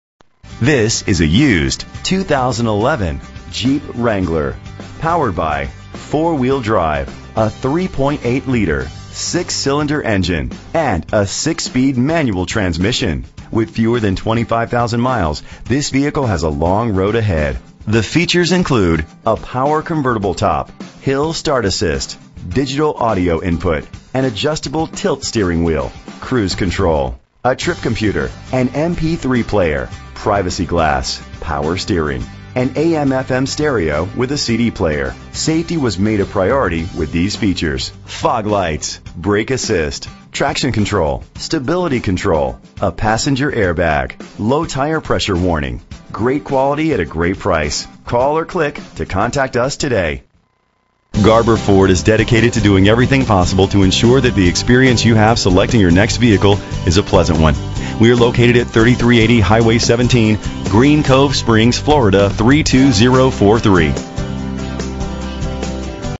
2011 Jeep Wrangler - Green Cove Springs FL
Phone:
Year: 2011
Make: Jeep
Model: Wrangler
Trim: Sport
Engine: 3.8 liter 6 cylinder 12 valve
Transmission: 6-Speed Manual
Color: Deep Cherry Red Crystal Pearlcoat
Mileage: 22357
Address:
3380 US HIGHWAY 17 S